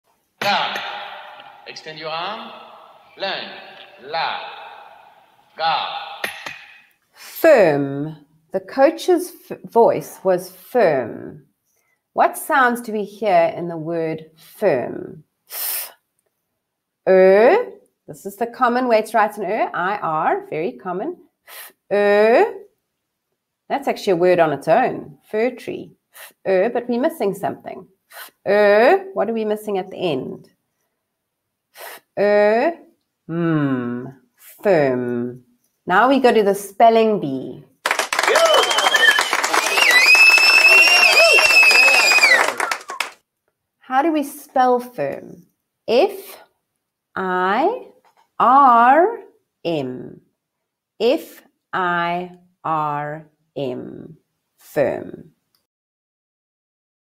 firm
Teaching the child how to sound out and spell the word firm correctly. (Please to find out more about WORDLIST WHIZZ)